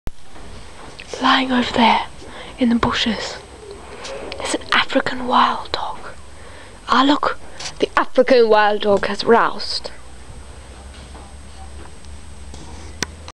The Incredible Wild - Part 5
An animal mocumentary I created last holidays with the help of my sister and cousin, not to mention all the animal stars!